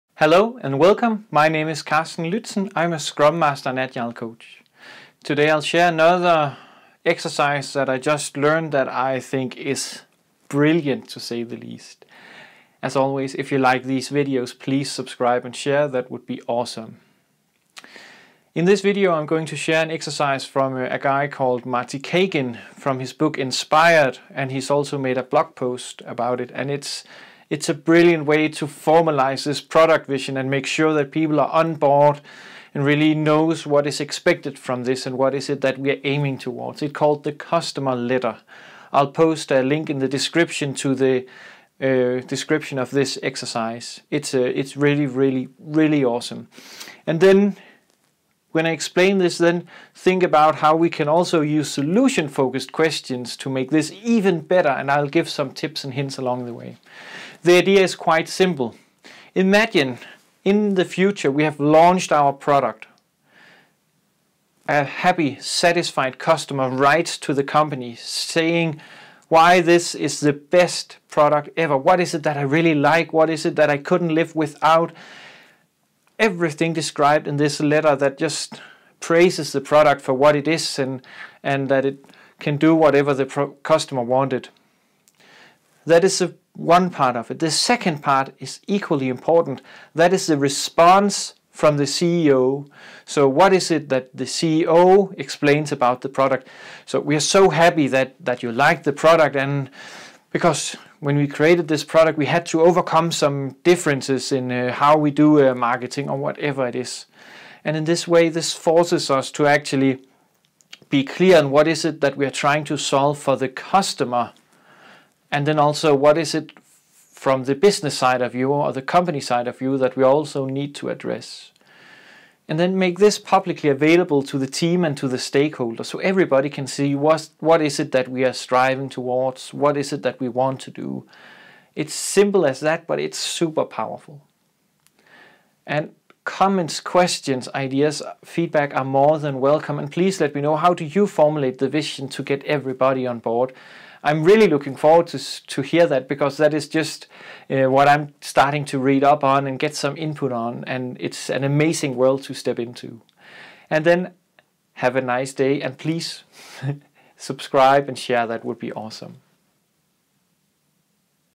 Customer Letter with Carsten Lützen
In this video I share an awesome exercise for helping to create a strong product vision. This one comes from Marty Cagan and is called: Customer Letter and it is brilliant!
It helps you to figure out what is important.

Comments, questions, ideas or feedback are more than welcome!